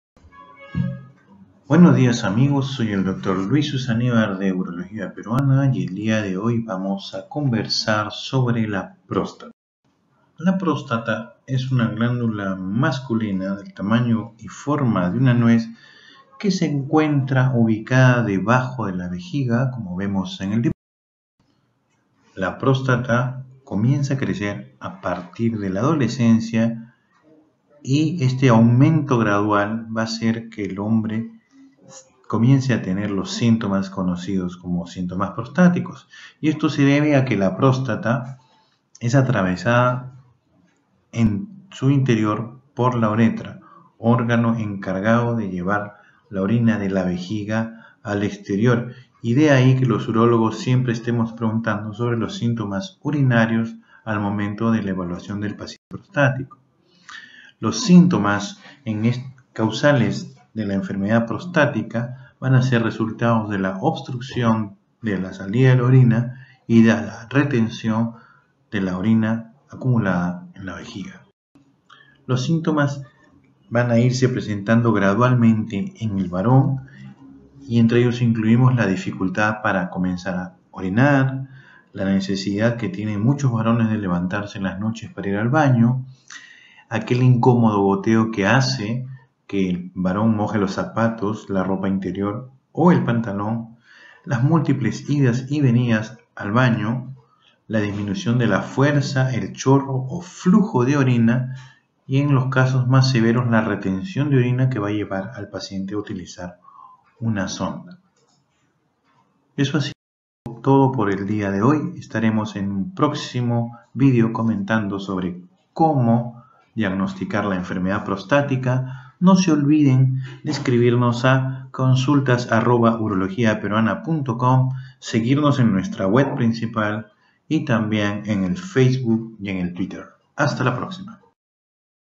La Próstata y sus síntomas. Urología Peruana: Dr. Luis Susanibar
Un vídeo donde se describen los principales síntomas de la próstata